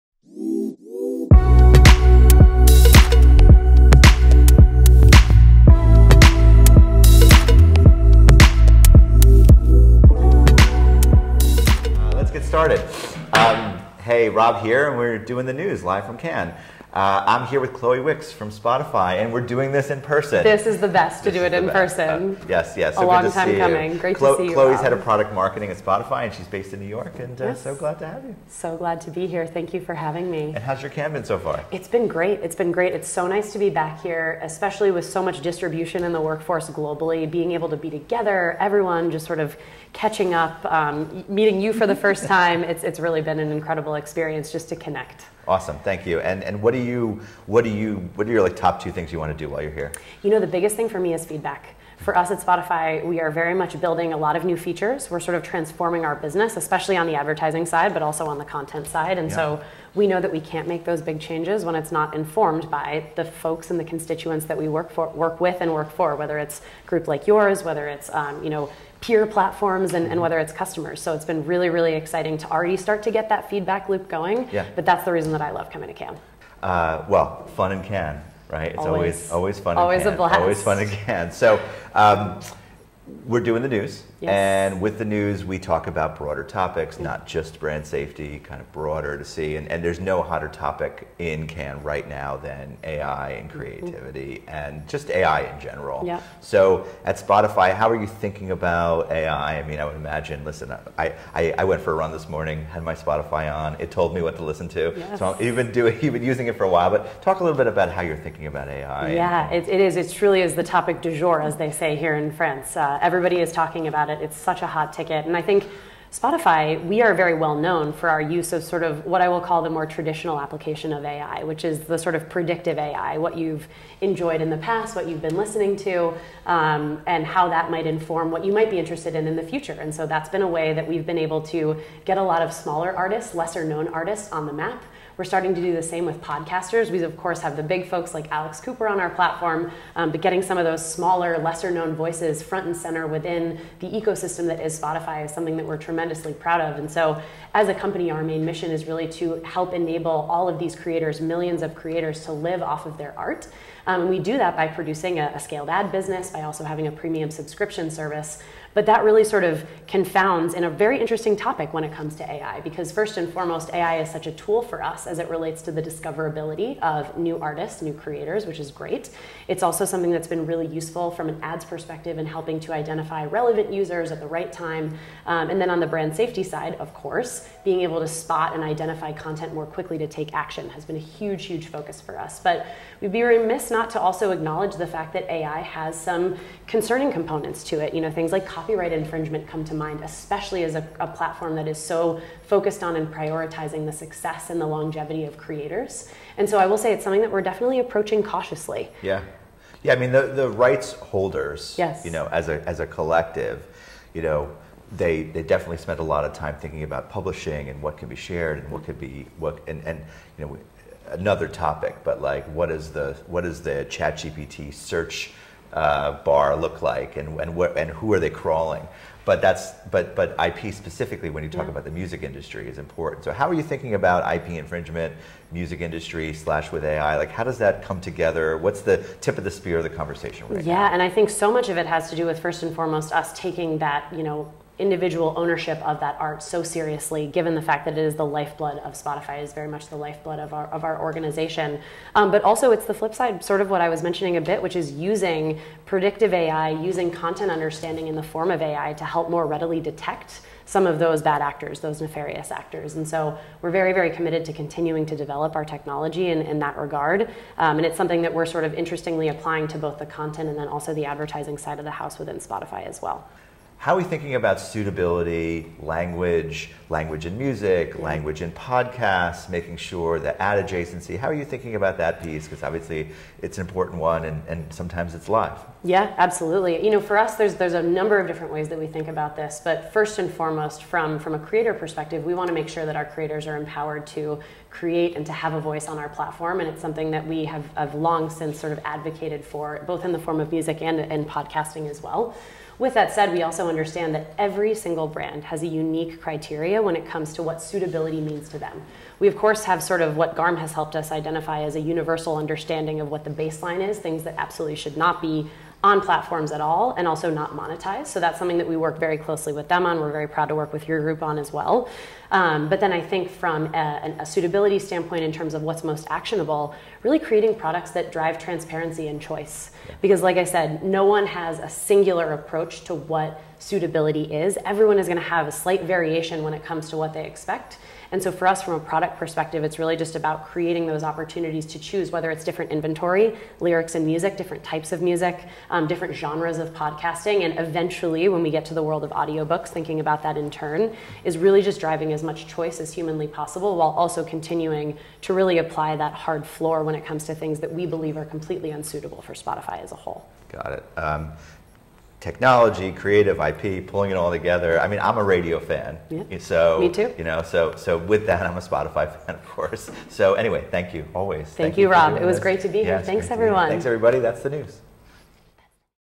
Industry News from Cannes 2023: Chloe Wix, Global Director, Monetization Product Marketing, Spotify
Chloe Wix, Global Director of Monetization Product Marketing at Spotify, discusses the role of AI in Spotify's operations, its impact on advertising and brand safety, and the challenges it presents, such as copyright and infringement issues. She also touches on Spotify's commitment to empowering creators and maintaining a platform that respects individual art ownership. She also delves into the company's approach to suitability and language in content and their plans for future product development. Tune in to learn more about Spotify's innovative strategies and their vision for the future.